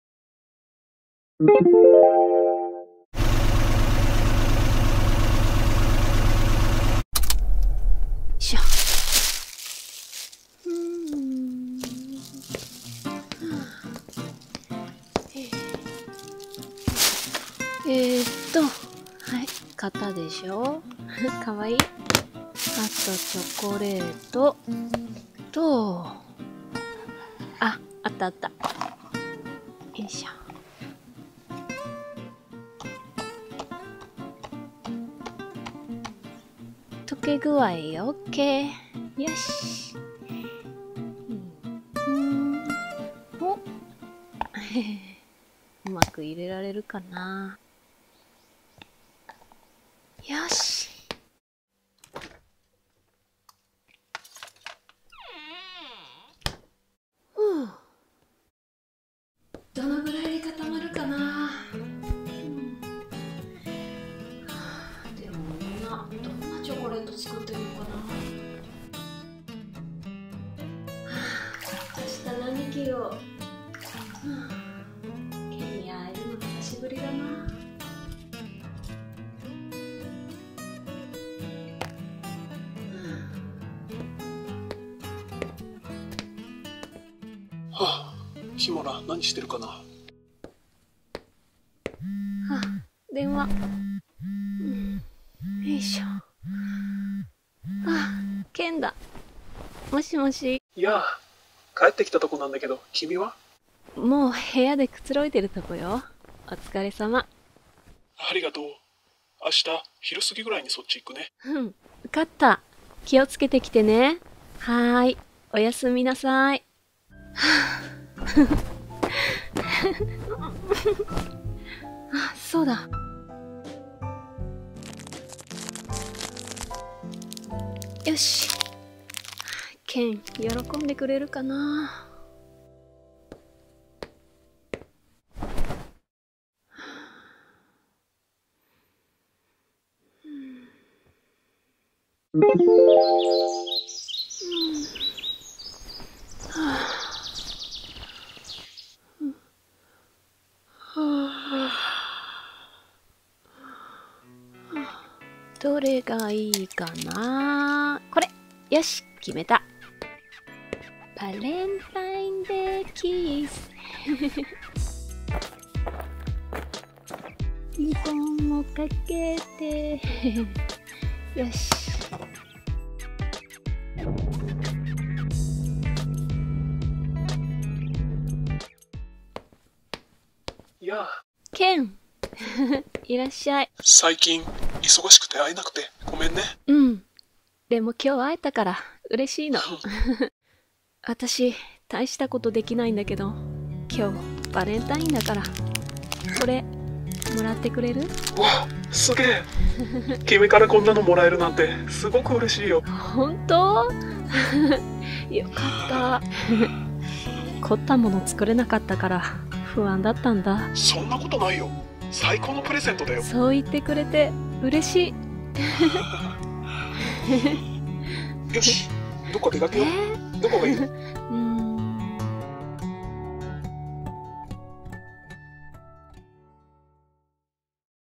japanese valentine day (Doll Movie)
Touch the subtitle mark to see English subtitles

日本のバレンタインデー☆
女の子からの手作りチョコレートを。

《使用ドール》
キモラ　Kimora lee simmons (barbie doll)
ケン　　I can be / Ice skater gift set (barbie doll)